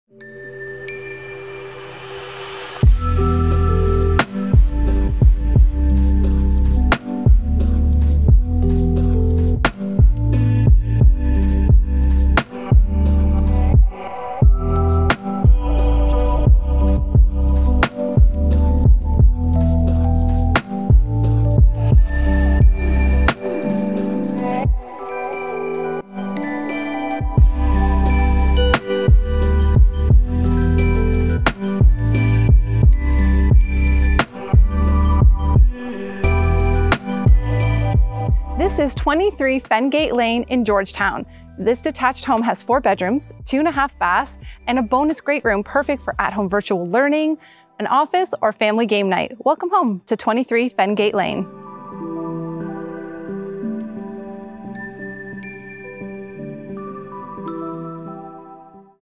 23 Fengate Lane | Georgetown
This luxurious house for sale in Georgetown has it all!
This property is absolutely stunning. From its beautifully landscaped 40 x 98 foot lot to the countless upgrades, this 2,620 square-foot home has it all.

Set in a location known for being family-friendly, this home is close to parks, schools, shopping, and highways. If convenience is important, it doesn't get much better than this.

Bonus Room!
One of our favourite features in this home is the mid-level family room; this rare feature is perfect for the entire family. Whether you use it for virtual learning or movie nights, this large yet cozy room is one you'll enjoy often.

The backyard is an extension of the living space that this home provides.  The fully landscaped yard includes a massive stone patio, a natural gas line for your BBQ, it's surrounded by gardens and finished off with a one-year-old hot tub and pergola.  Whether warm Summer afternoons or cool Winter evenings, you'll love spending time here.

We love an upgraded home!
Be sure to check out the full list of features in the brochure, as there are far too many to mention here. There is nothing we would change, inside or out. This property has been meticulously maintained, it's ready to move in and enjoy, and It could be yours - come see it today!